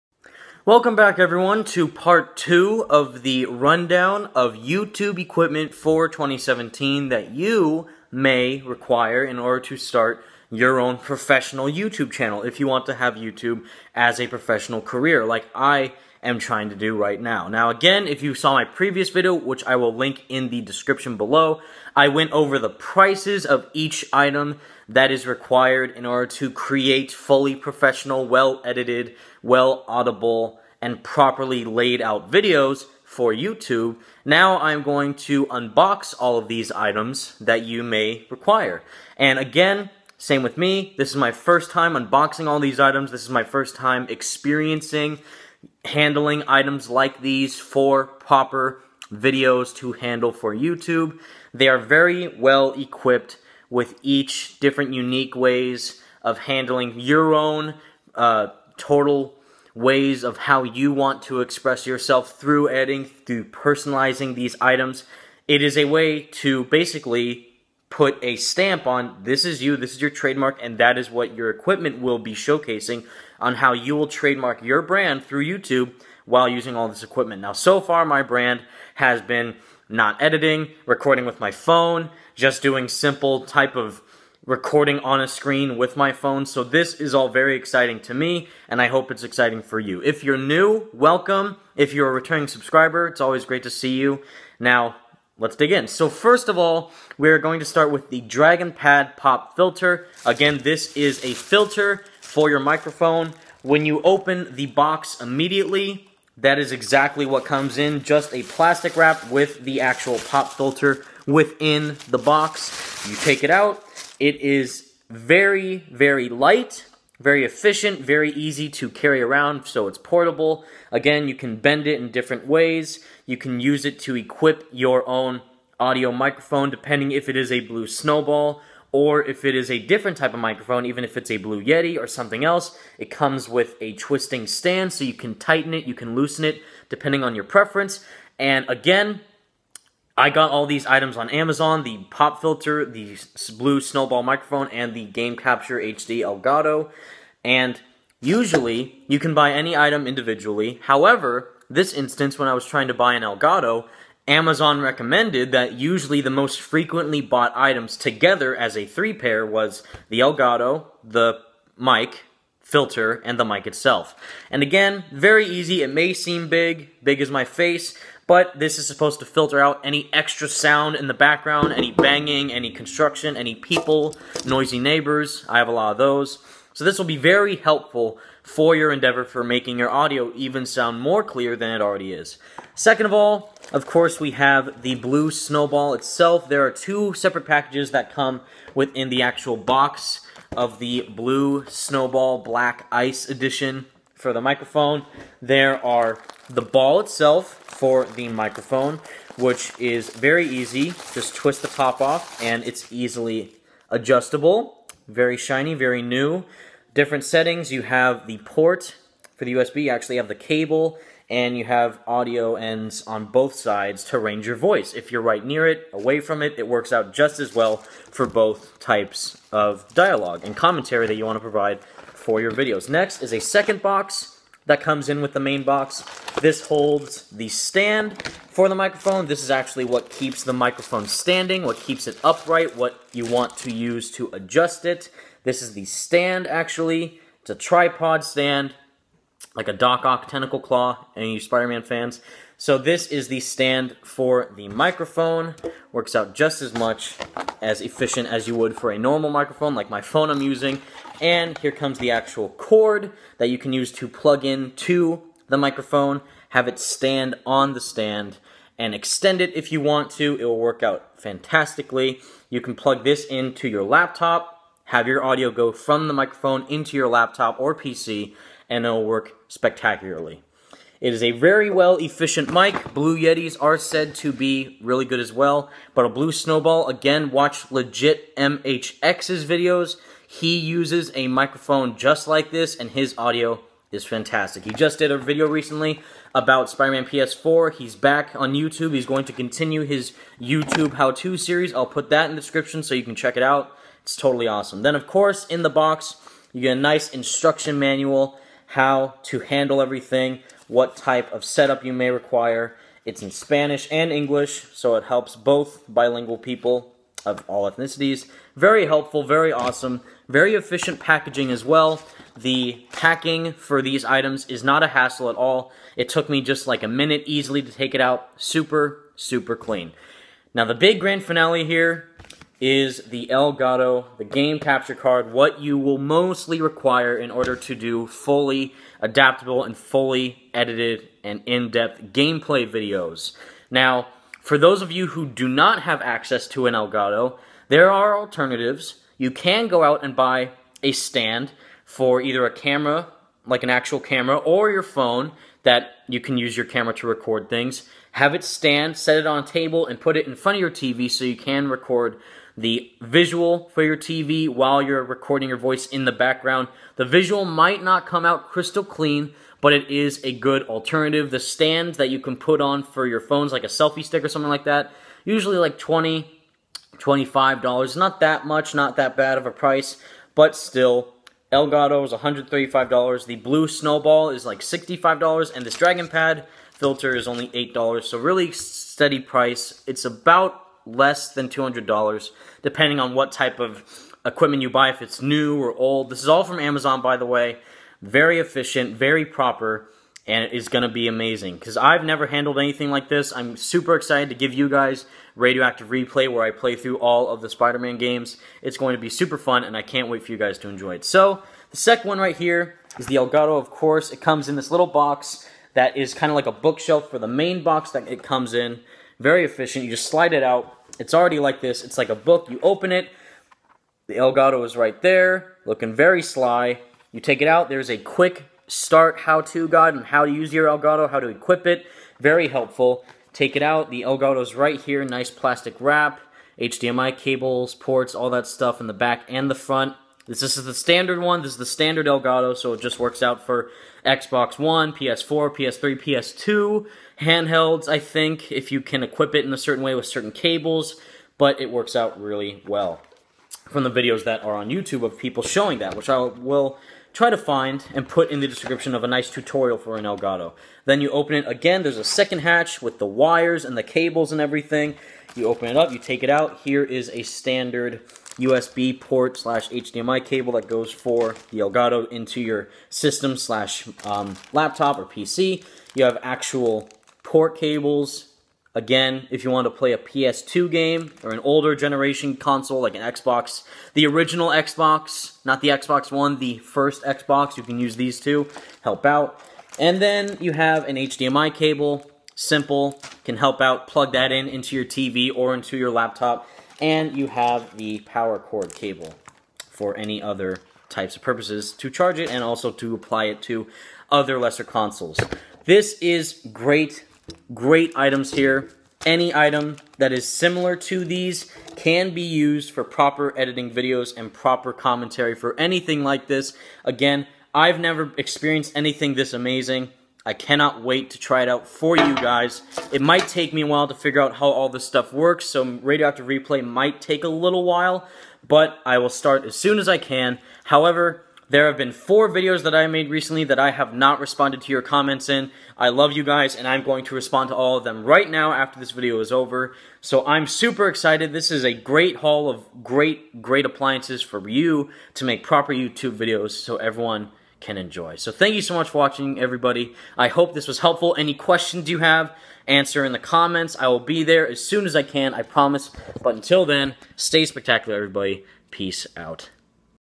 Elgato Game Capture HD, Blue Snowball Ice (Black) Microphone, & DragonPad Pop Filter Unboxing
In this video I unbox and overview what equipment YOU will need in order to be a YouTuber as a full-time, financially sustainable career.

DragonPad Pop Filter

Blue Snowball Ice Mic (Black Edition)

Elgato Game Capture HD